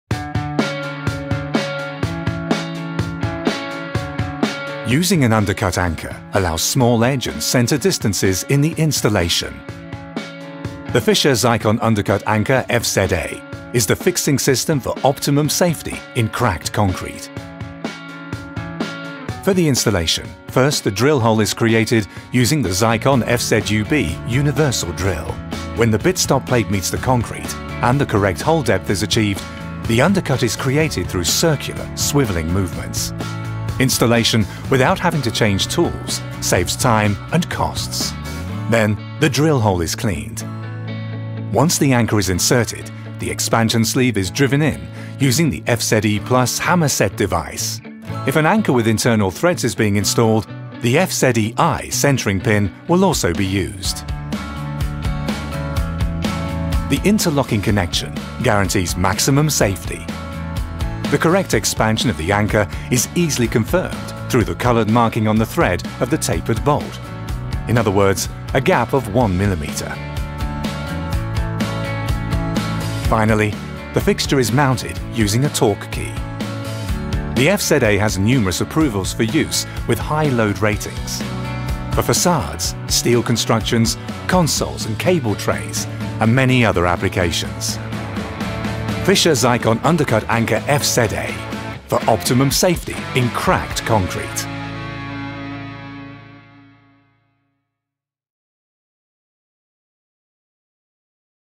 fischer FZA ZYKON undercut anchor - The fixing system with the highest safety in cracked concrete
The fischer ZYKON undercut anchor FZA is an anchor made from zinc-plated steel, stainless steel and highly corrosion-resistant steel. The undercut anchor is ideal for anchoring façades, railings and steel constructions in cracked concrete, both inside and outside. The undercut drill hole is created in a single work step with the special drill bit FZUB. The undercut anchor is set in a pre-positioned and push-through installation. With the setting tool FZE Plus, the expansion sleeve is pressed over the cone and fills the undercut drill hole in a positive-locking manner. This ensures top safety even for large crack widths. The European Technical Approval and classification into fire resistance class R 120 ensure added safety.